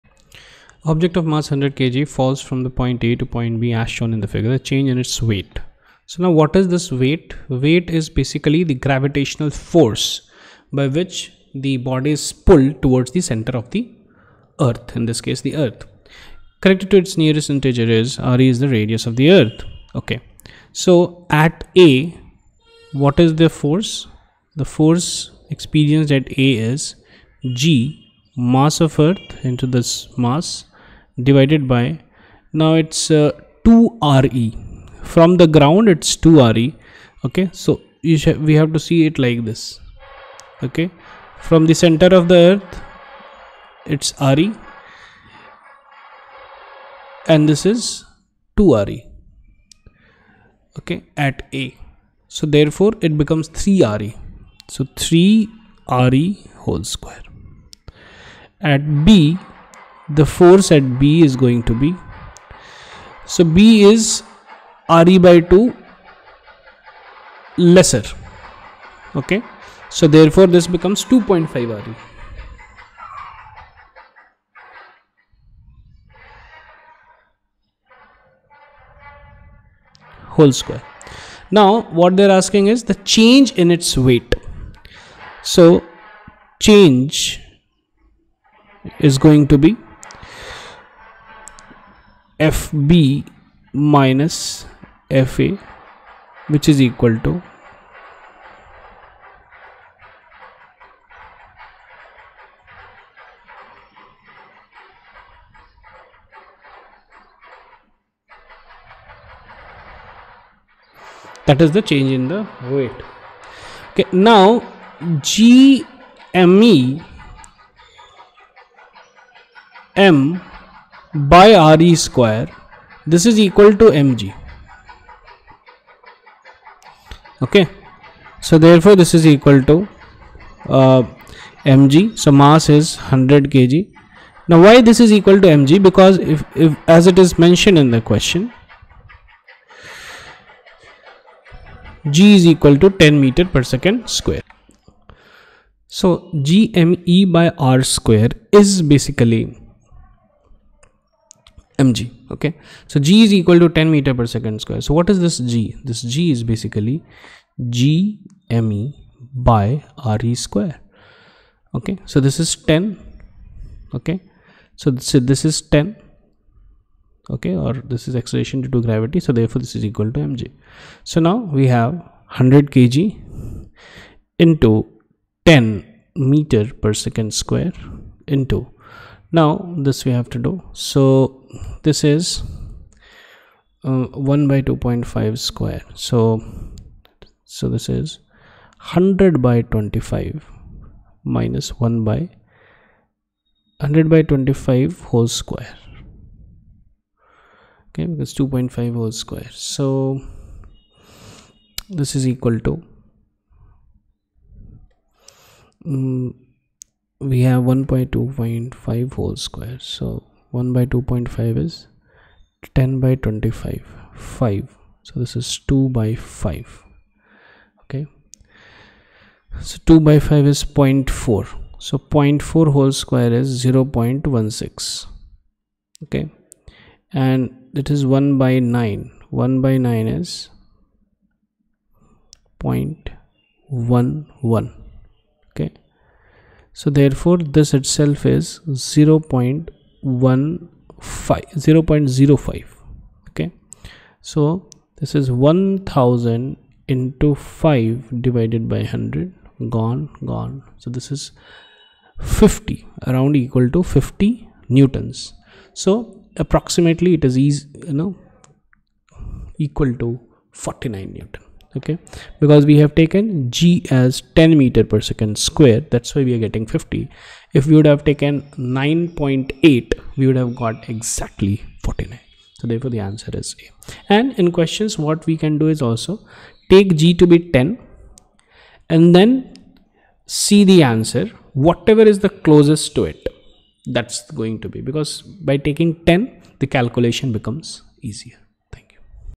An object of mass 100 kg falls from point A to B as shown in figure. The change in its weight,
An object of mass 100 kg falls from point  A  to  B  as shown in figure. The change in its weight, corrected to the nearest integer is ( 𝑅_𝐸  is the radius of the earth)
        NEET 2024
A) 49 N
B) 89 N
C) 5 N
D) 10 N